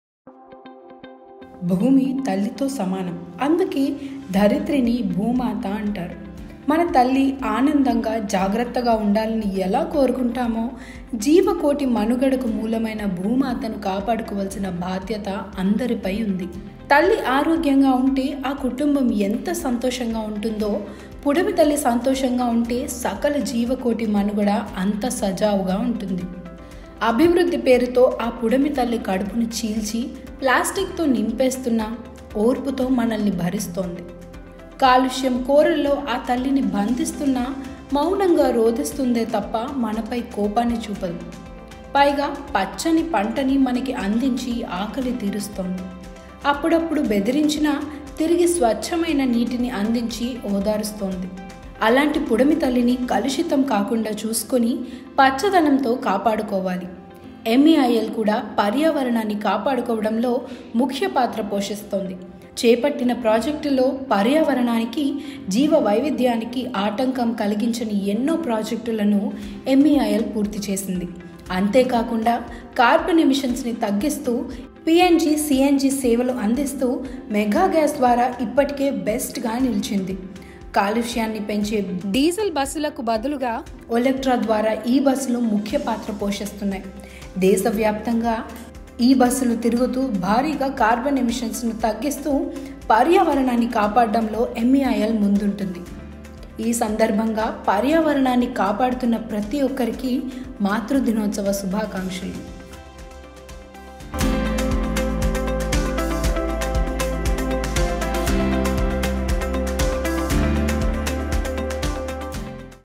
మాతృమూర్తికి తోడుగా MEIL
భూమి తల్లితో సమానం. అందుకే ధరిత్రిని భూమాత అంటారు. మన తల్లి ఆనందంగా, జాగ్రత్తగా  ఉండాలని ఎలా కోరుకుంటామో.. జీవకోటి మనుగడకు మూలమైన భూమాతను కాపాడుకోవాల్సిన బాధ్యత అందరిపై ఉంది. తల్లి ఆరోగ్యంగా ఉంటే ఆ కుటుంబం ఎంత సంతోషంగా ఉంటుందో.. పుడమి తల్లి సంతోషంగా ఉంటే సకల జీవకోటి మనుగడ అంత సజావుగా ఉంటుంది.
అభివృద్ధి పేరుతో ఆ పుడమి తల్లి కడుపును చీల్చి, ప్లాస్టిక్తో నింపేస్తున్నా.. ఓర్పుతో మనల్ని భరిస్తుంది. కాలుష్య కోరల్లో ఆ తల్లిని బంధిస్తున్నా.. మౌనంగా రోధిస్తుందే తప్పా.. మనపై కోపాన్ని చూపదు. పైగా పచ్చని పంటను మనకు అందించి ఆకలి తీరుస్తుంది. అప్పుడప్పుడు బెదిరించినా.. తిరిగి స్వచ్ఛమైన నీటిని అందించి ఓదార్చుతుంది. అలాంటి పుడమి తల్లిని కలుషితం కాకుండా చూసుకుని పచ్చదనంతో కాపాడుకోవాలి.

MEIL కూడా పర్యావరణాన్ని కాపాడుకోవడంలో ముఖ్యపాత్ర పోషిస్తోంది. చేపట్టిన ప్రాజెక్టుల్లో పర్యావరణానికి, జీవవైధ్యానికి ఆటంకం కలిగించని ఎన్నో ప్రాజెక్టులను MEIL పూర్తి చేసింది. అంతేకాకుండా కర్బన్ ఎమిషన్స్ను తగ్గిస్తూ.. PNG,CNG సేవలు అందిస్తూ ఇప్పటికే బెస్ట్గా నిలిచింది. కాలుష్యాన్ని పెంచే డీజిల్ వాసులకు బదులుగా ఒలెక్ట్రా ద్వారా EV బస్సులు ముఖ్యపాత్ర పోషిస్తున్నాయి. దేశవ్యాప్తంగా ఈ బస్సులు తిరుగుతూ భారీగా కర్బన్ ఎమిషన్స్ను తగ్గిస్తూ పర్యావరణాన్ని కాపాడడంలో MEIL ముందుంటుంది. ఈ మాతృదినోత్సవం రోజున.. పర్యావరణాన్ని కాపాడుతున్న ప్రతి ఒక్కరికి మాతృదినోత్సవ శుభాకాంక్షలు.